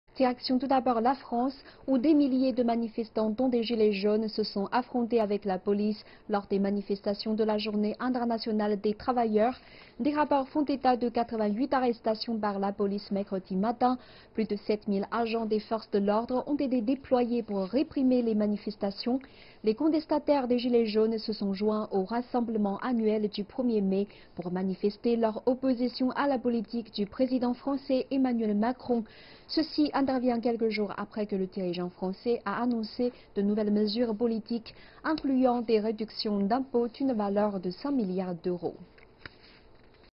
France : des Gilets jaunes mobilisés pour la Journée internationale des travailleurs
En France, des milliers de manifestants dont des Gilets jaunes se sont affrontés avec la police lors des manifestations de la Journée internationale des travailleurs. Des rapports font état de 88 arrestations par la police mercredi matin. Plus de 7 mille agents des forces de l'ordre ont été déployés pour réprimer les manifestations. Les contestataires des Gilets jaunes se sont joints aux rassemblements annuels du 1er mai pour manifester leur opposition à la politique du président français Macron. Ceci intervient quelques jours après que le dirigeant français a annoncé de nouvelles mesures politiques, incluant des réductions d'impôt d'une valeur de 5 milliards d'euros.